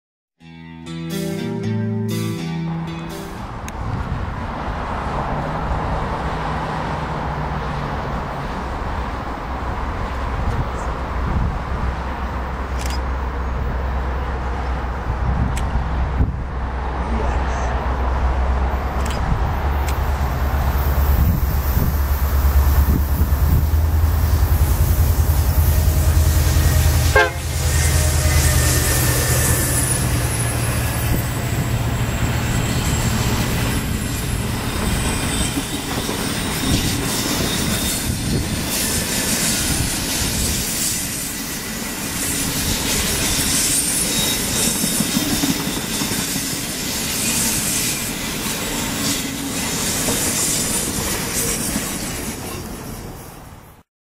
VIA 14 With Coors Light 6408 - July 5, 2011
VIA 6408 (Finally got her on video) leads train 14 past Birch Cove as they near their final destination of downtown Halifax, NS.  Enjoy!